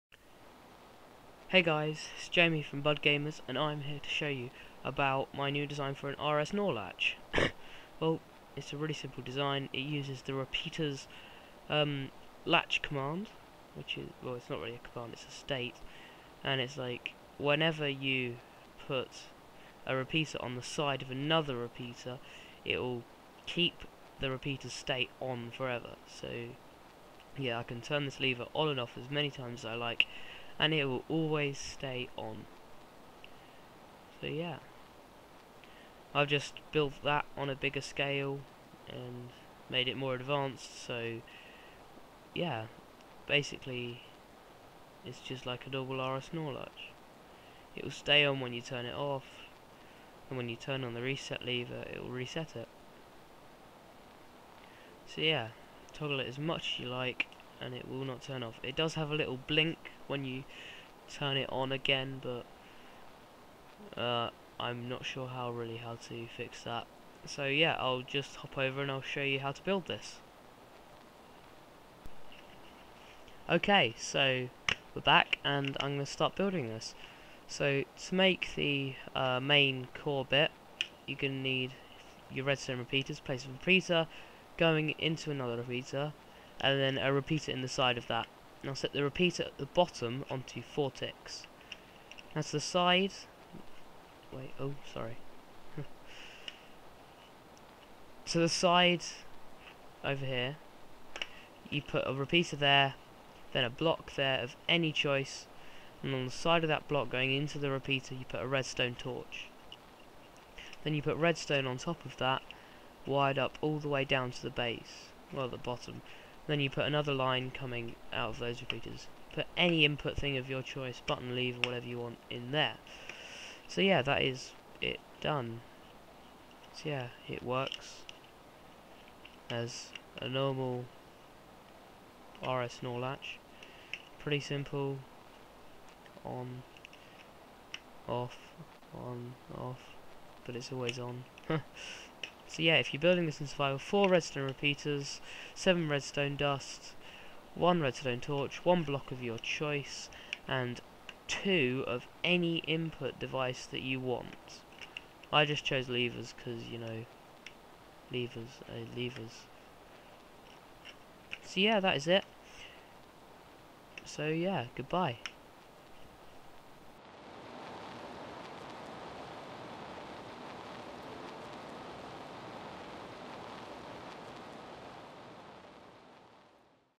Redstone Tutorials - Compact RS NOR Latch
Today I show you how to make a compact RS NOR Latch, in preparation for my upcoming Redstone Computer series